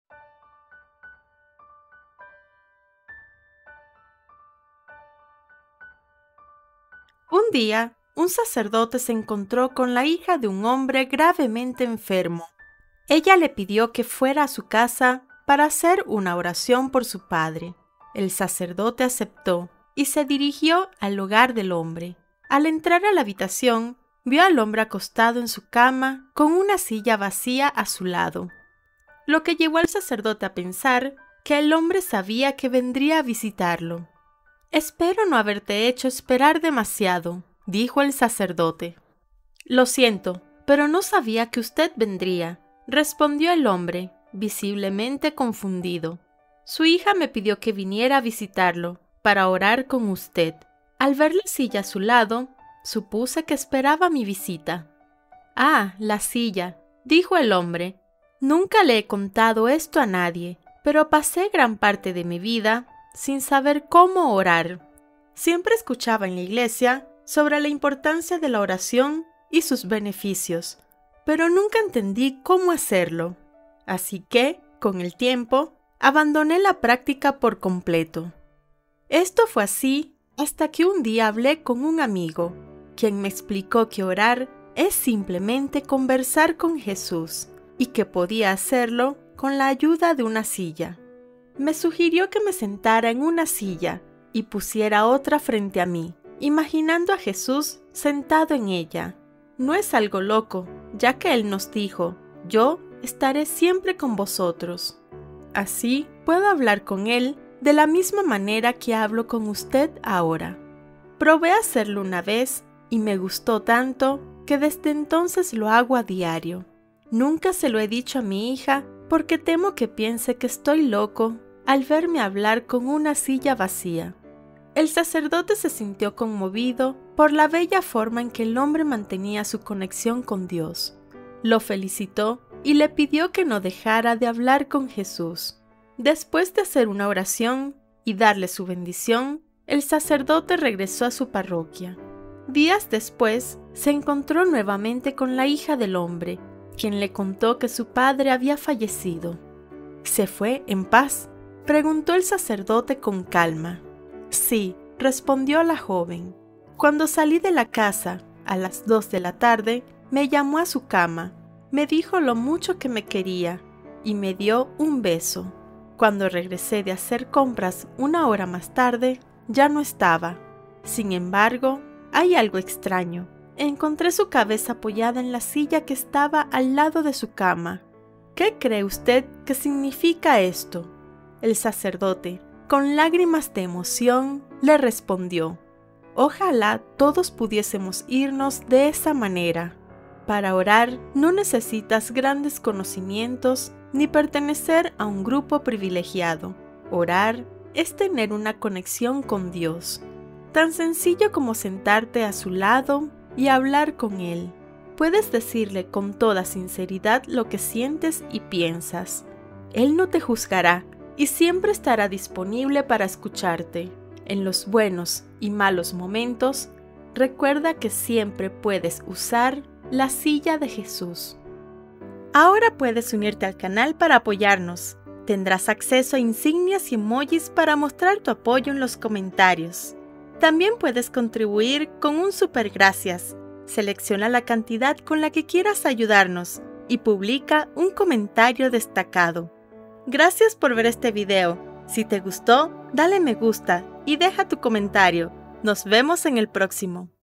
Descubre el Secreto de la Oración con la Silla de Jesús
En este conmovedor video, un sacerdote se encuentra con una joven que le pide visitar a su padre gravemente enfermo. Al llegar, el sacerdote descubre una silla vacía junto a la cama del hombre, un objeto que guarda un profundo significado. A través de una conversación sincera, el hombre revela su singular manera de orar: imaginando a Jesús sentado en esa silla, como un amigo cercano con quien compartir sus pensamientos y sentimientos. Este hermoso acto de fe y conexión con Dios conmueve al sacerdote, quien lo alienta a seguir hablando con Jesús.

Días después, el sacerdote se entera de la muerte del hombre. A pesar de la tristeza, encuentra consuelo al saber que el hombre partió en paz, apoyando su cabeza en la silla, como si se despidiera de un amigo querido. Esta historia nos recuerda que la oración es una conversación íntima con Dios, accesible para todos, y que siempre podemos recurrir a la "silla de Jesús" para buscar consuelo y guía.